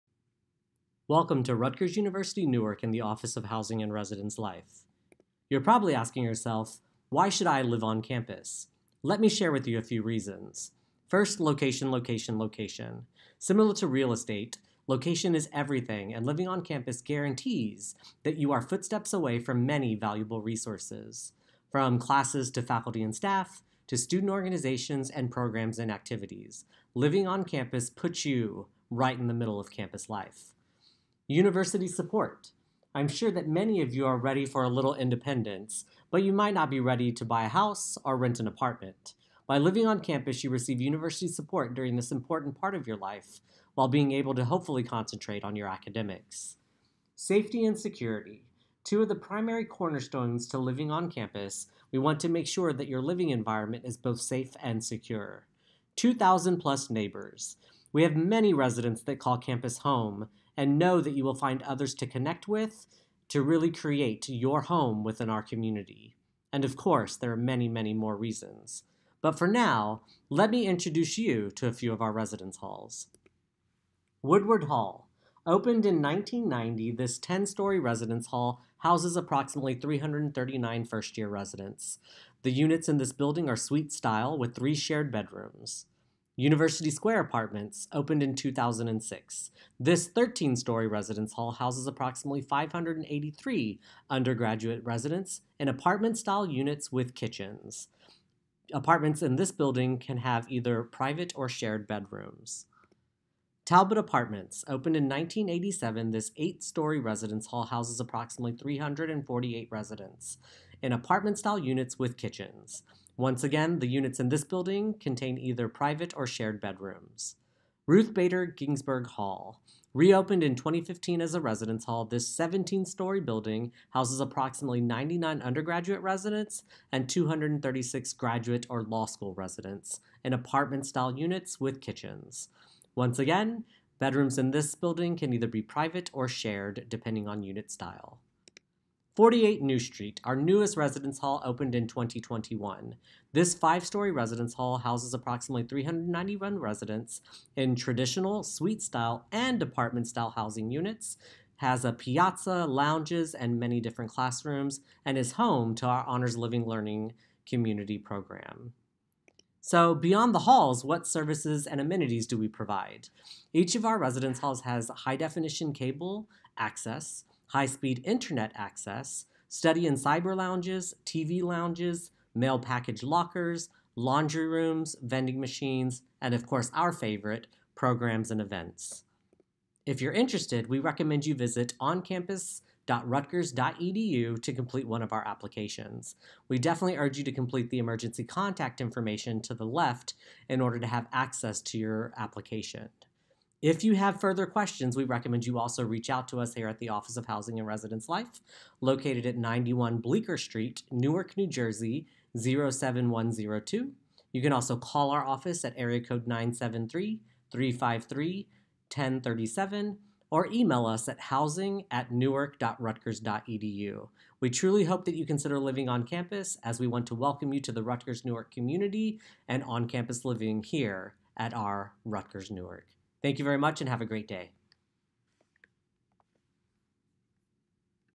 Rutgers University - Newark Housing and Residence Life Introduction
An Introduction to the Office of Housing and Residence Life at Rutgers University - Newark, including why one should consider living on-campus and brief overviews of each residence hall.